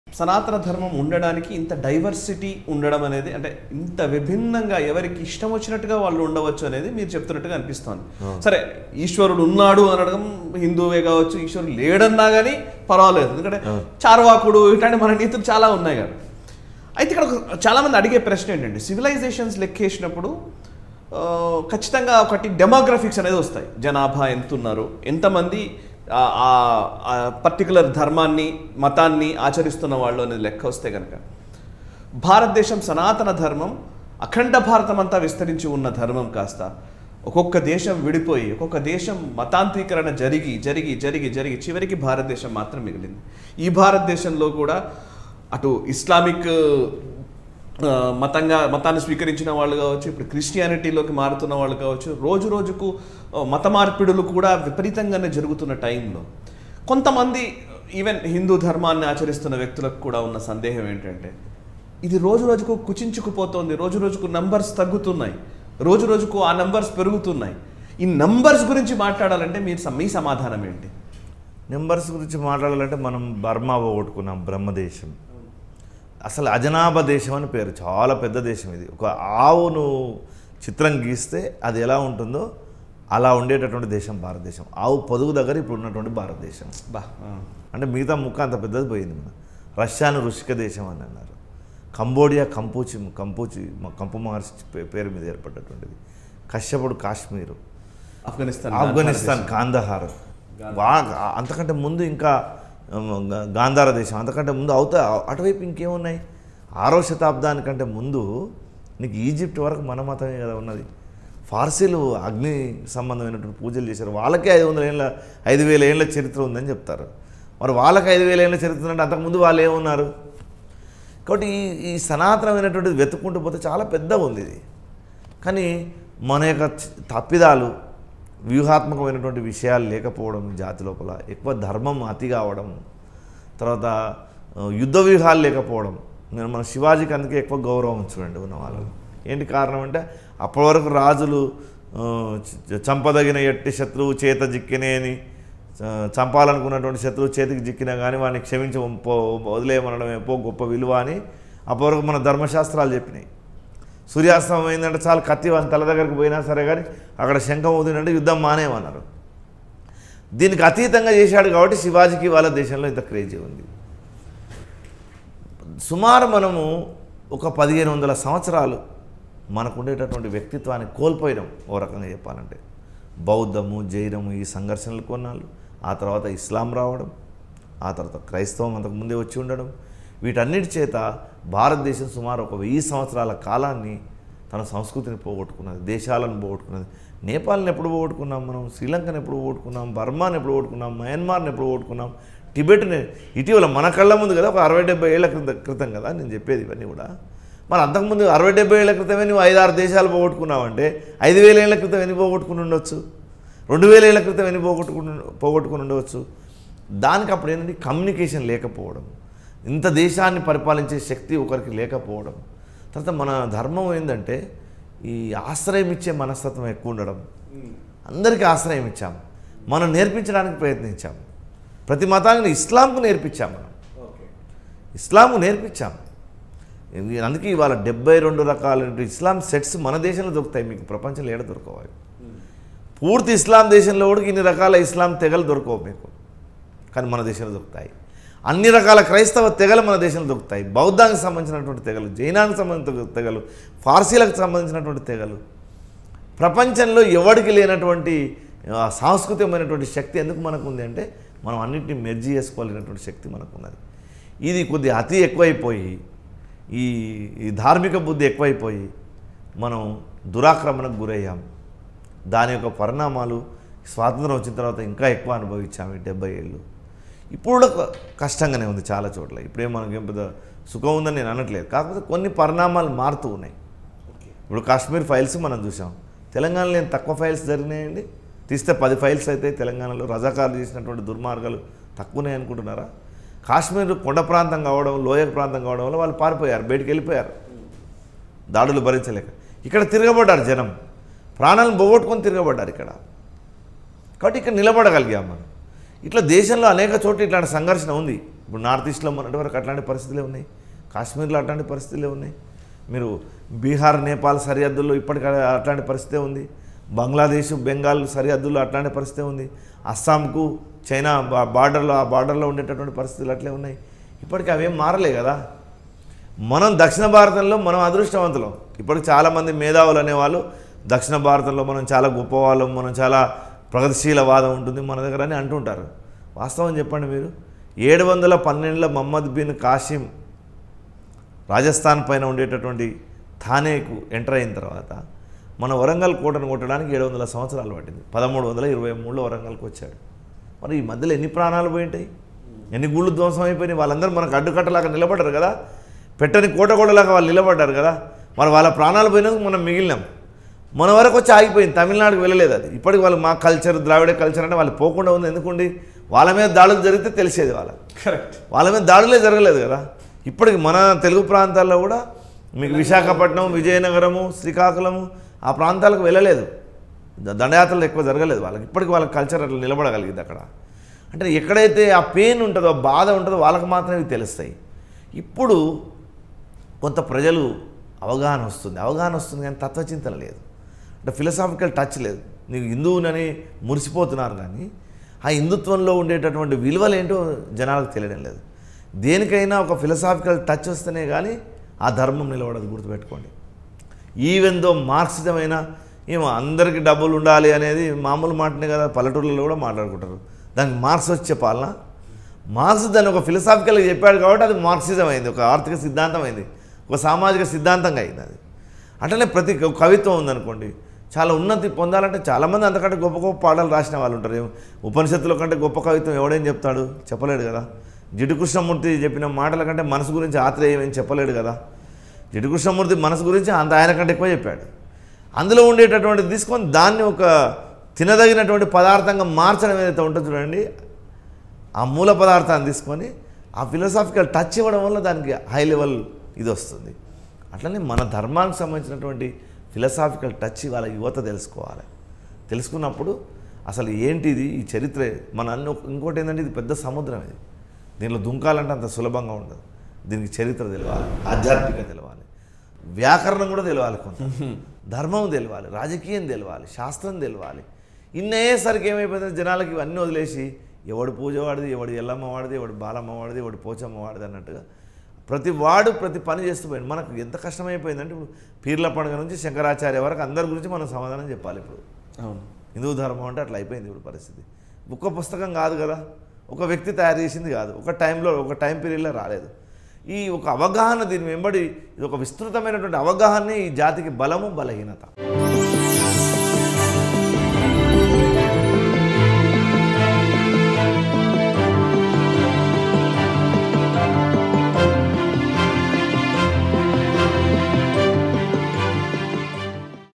Bhaskara Yogi About Sanatana Dharma | Special Discussion | Sai Krishna | Nationalist Hub
Our Company account details :
Name: KALEIDO FRAMES LLP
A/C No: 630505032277
IFSC Code : ICIC0006305